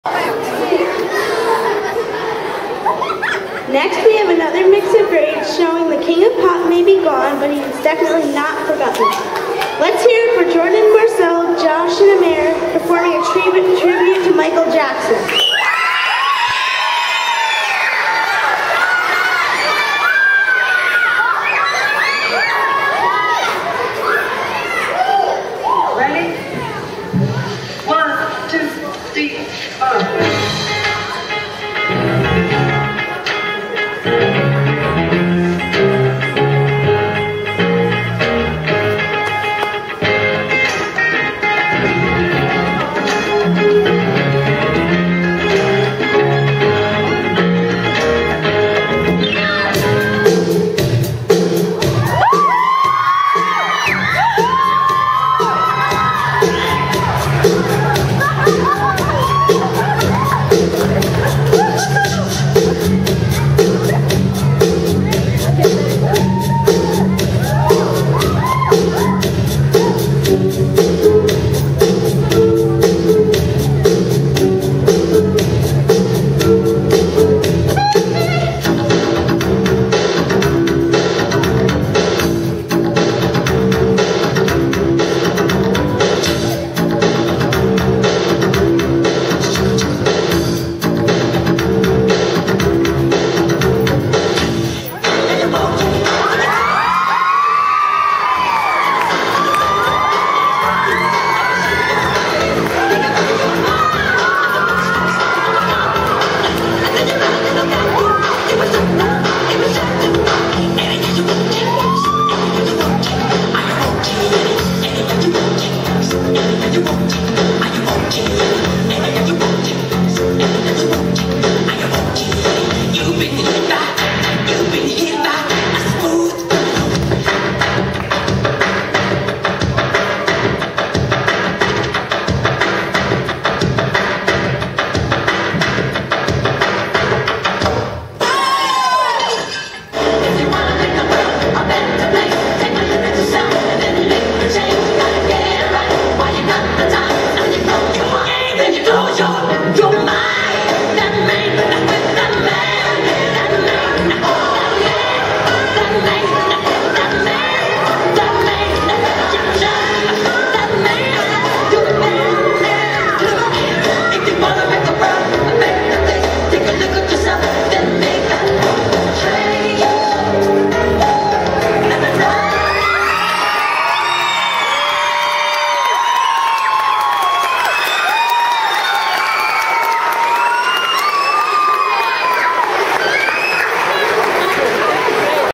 Variety Show 2010 - Michael Jackson Tribute
The kids are back at it again. Marcel & Jordan have enlisted friends Amer and Josh to perform a tribute to the King of Pop. Enjoy!